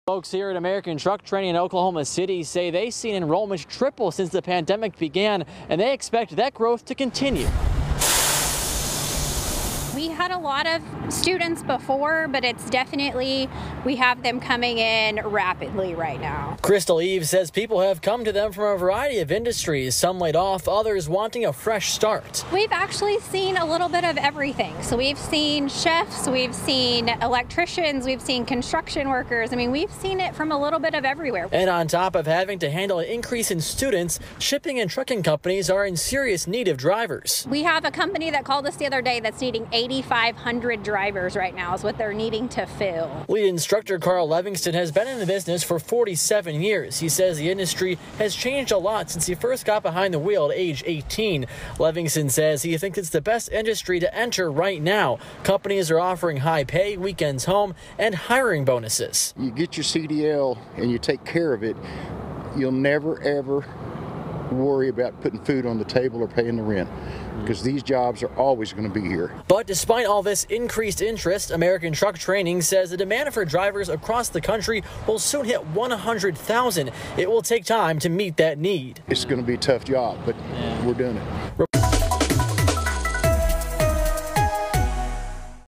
Oklahoma City truck driving school sees progress amidst supply chain issues
Though many industries are seeing supply chain issues, one OKC truck driving school has seen gradual progress toward fixing those issues.